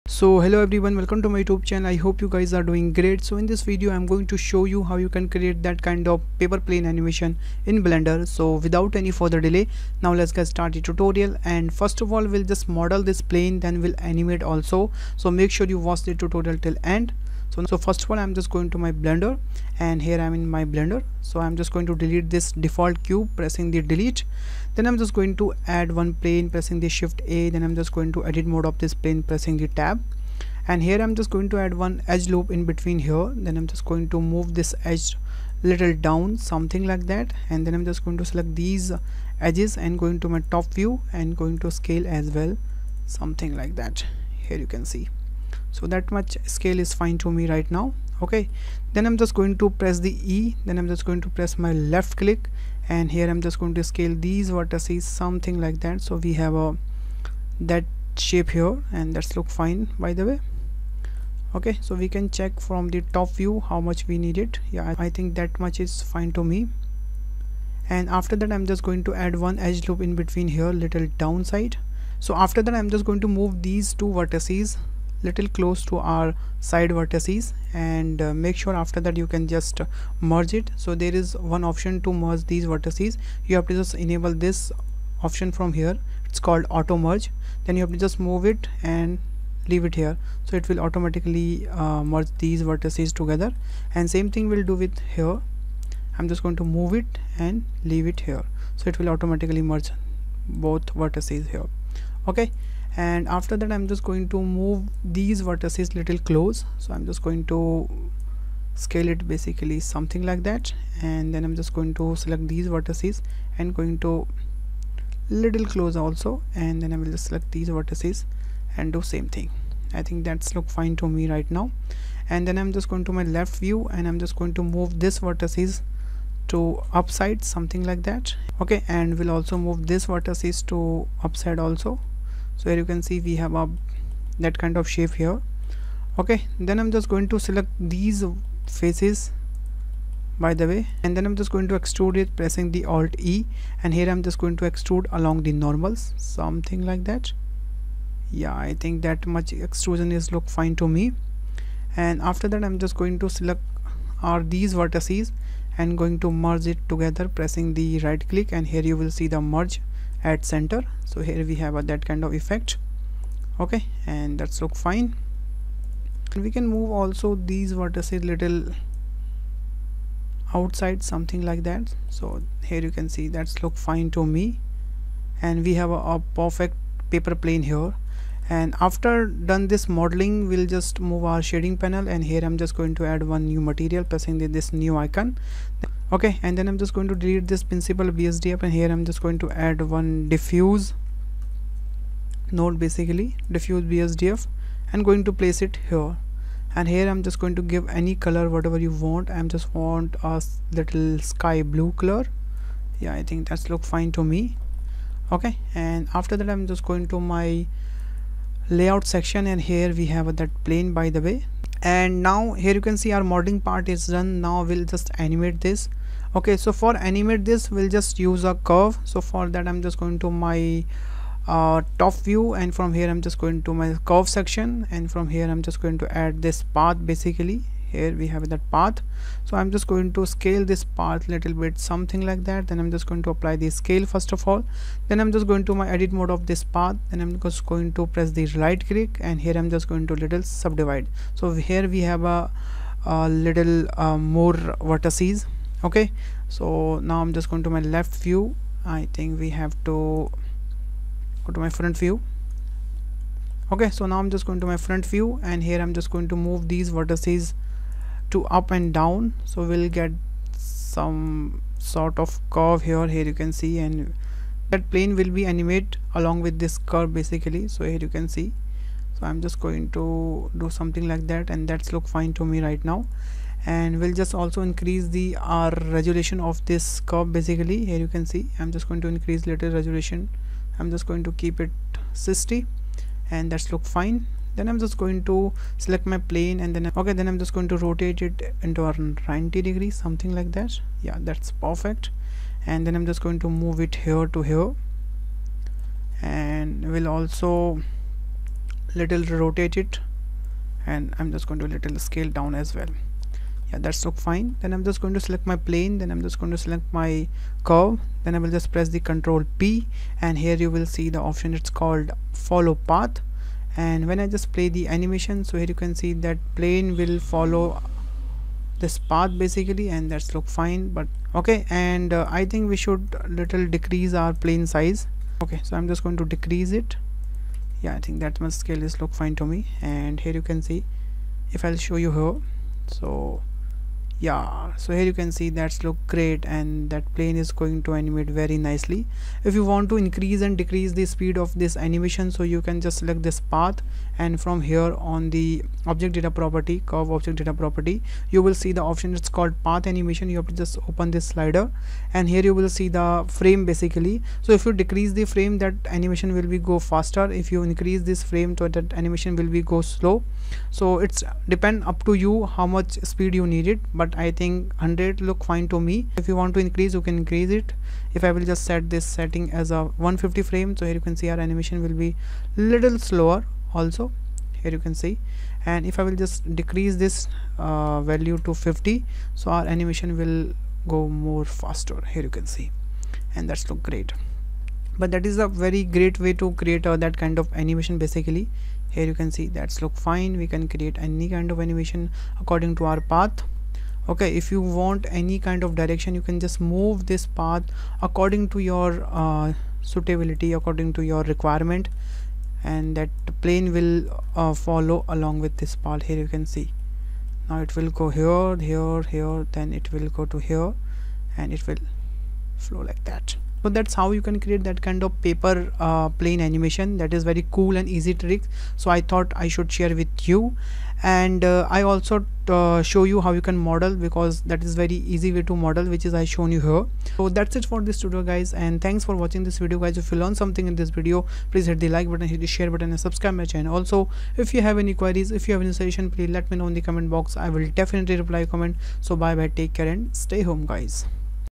Let's Do A Paper Plane Animation
Hello, guys, I hope you are doing well and in this video, I'm going to show you how you can create a paper plane and also show you how you can animate in Blender and I hope you will like this tutorial if you like the video make sure to hit the subscribe button and hit the bell icon too.